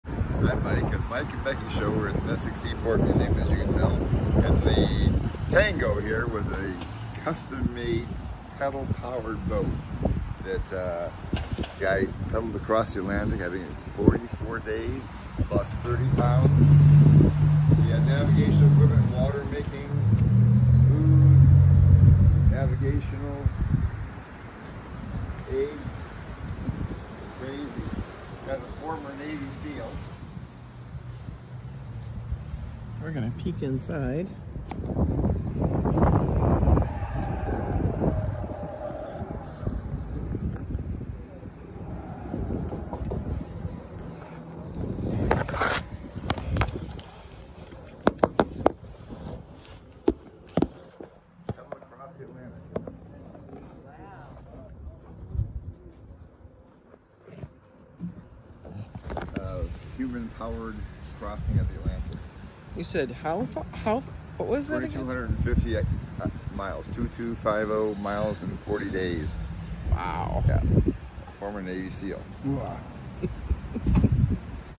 TMABS - the Tango, petal-powered boat
The Tango, a petal-powered boat that crossed the Atlantic in 40 days. Views inside boat are from the side.